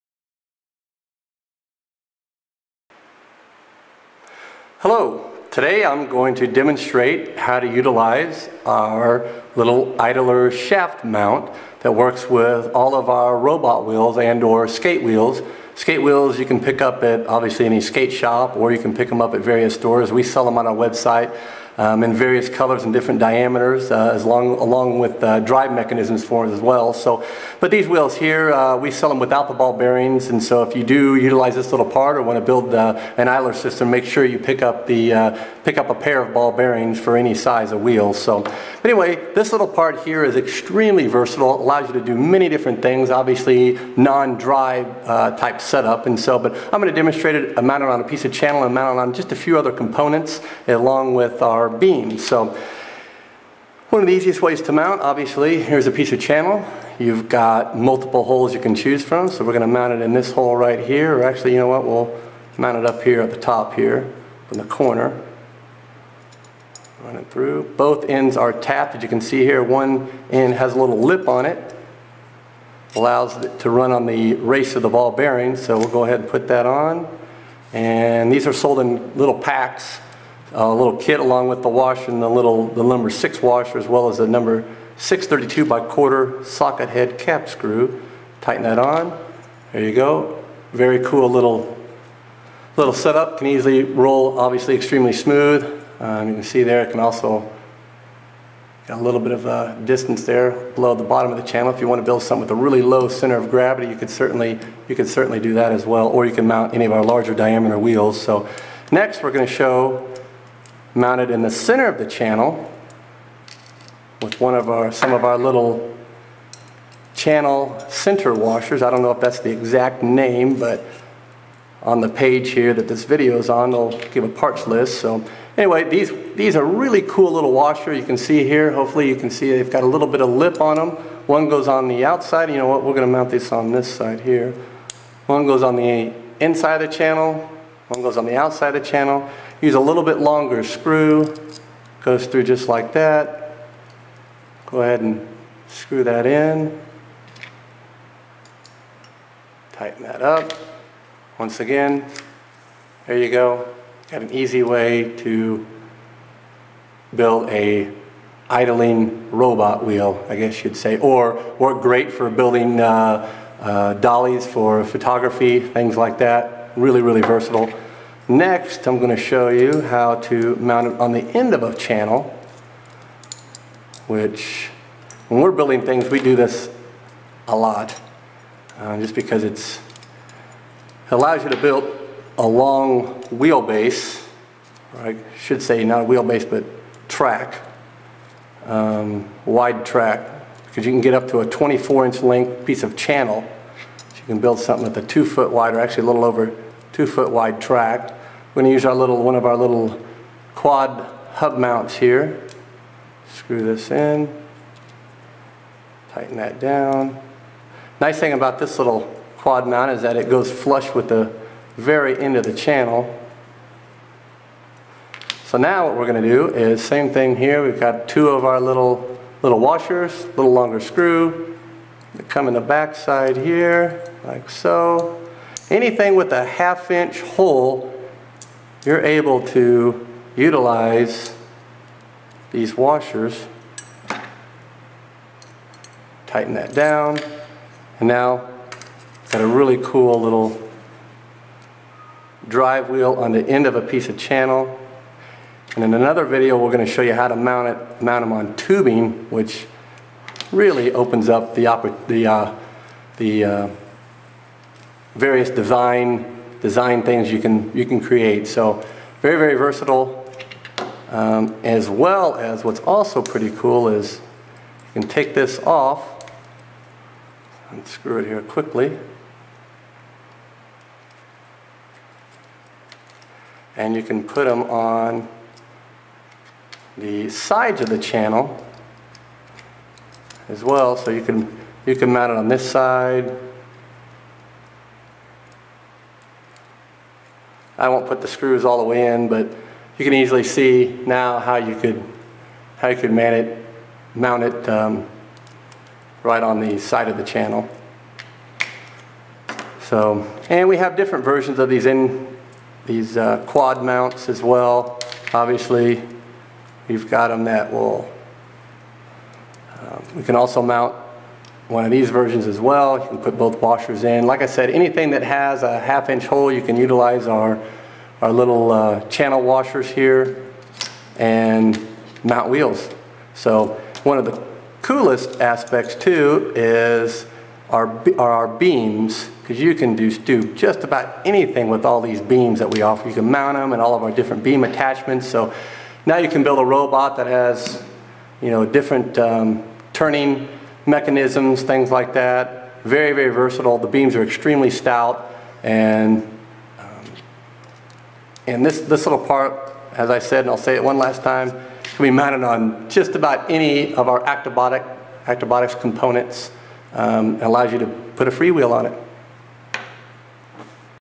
Actobotics™ - Mounting Skate Wheels using 8mm Flanged Standoff (SKU: 585488)
This video will show you various ways to mount Actobotics™ low friction skate wheels to other components using part number 585488.